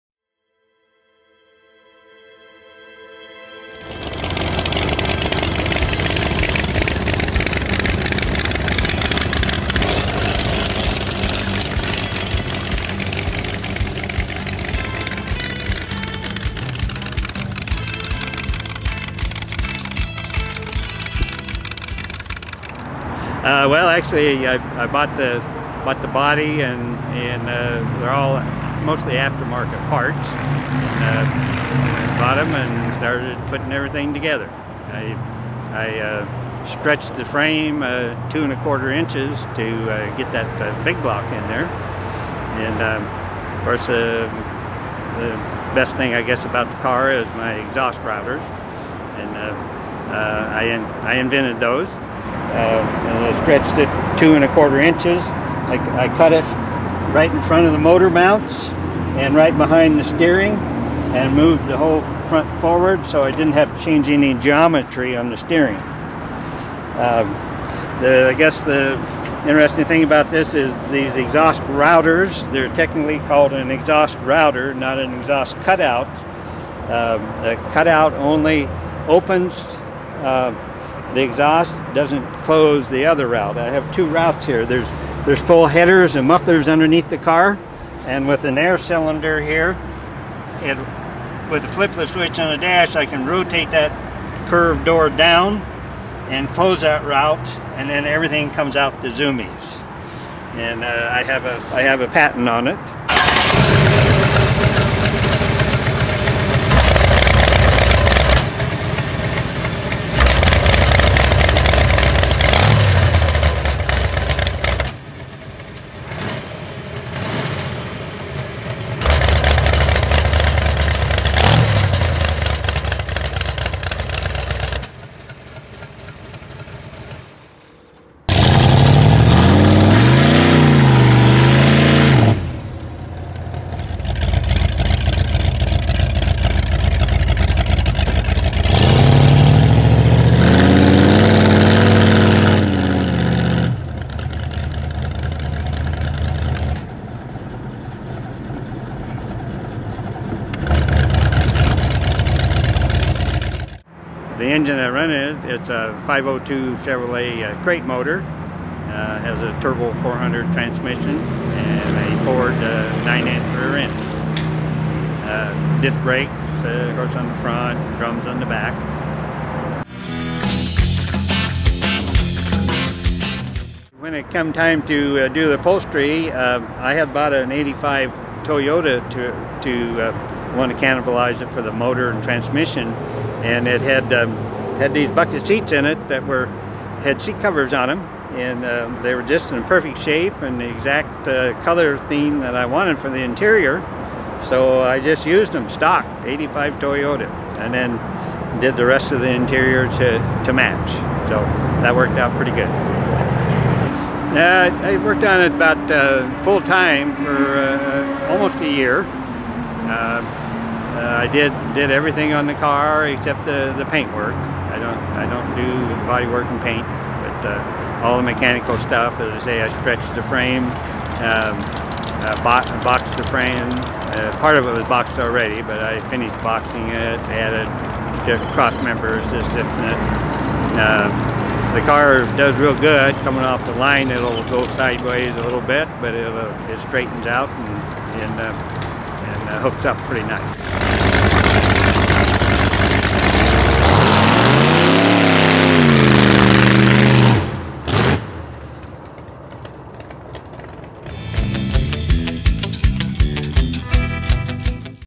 ONLINE AUTOMOTIVE MUSEUM - 1934 Ford
Denny Wirt brought a beautiful 1934 Ford to the Cars and Coffee  Las Vegas Show Saturday.  Denny not only built the Ford but also fabricated a unique exhaust flow system of which he owns a Patent .